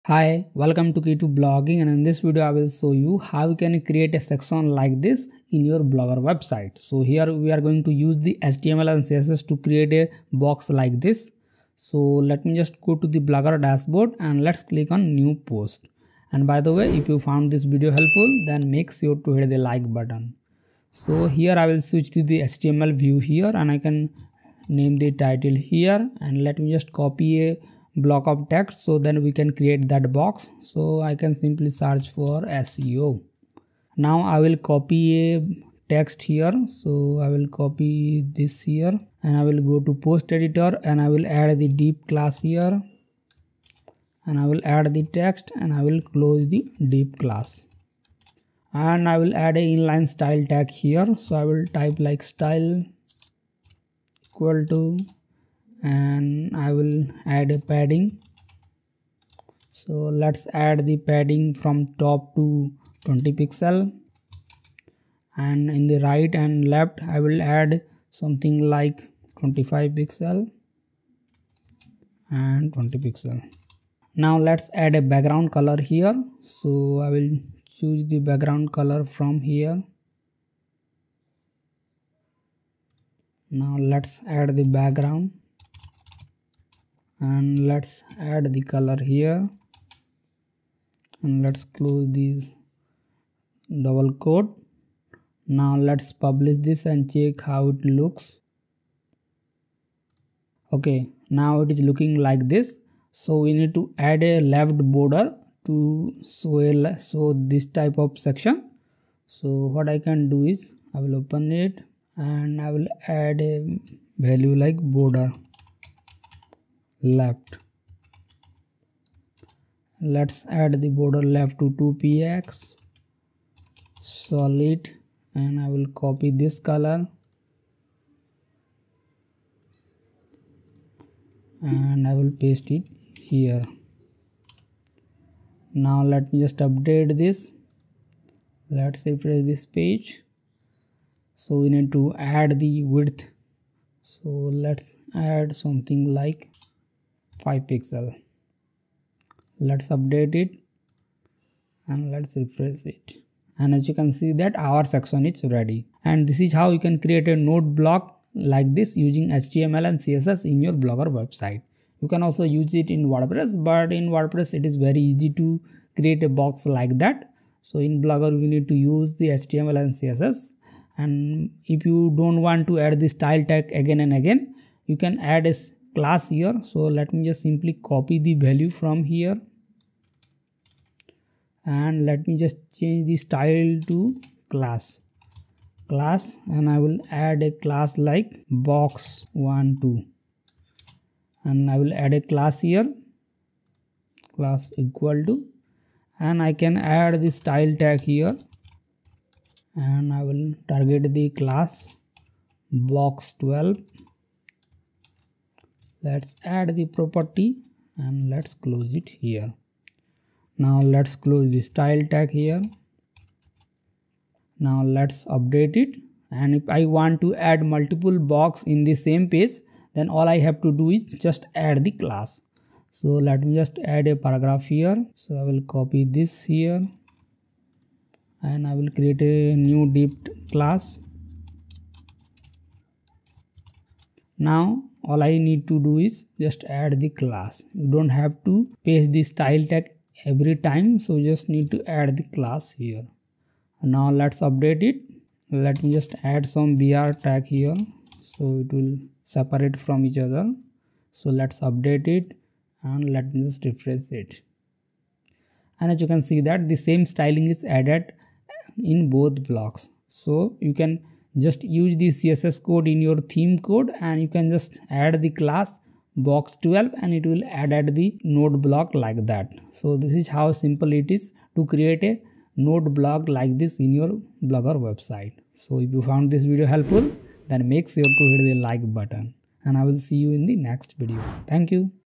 Create Notebox using HTML & CSS ⚡⚡
In this video, I will show you how to create a Beautiful Notebox using HTML & CSS only. Follow the video to create a similar Note Block for your Blogger and Wordpress websites.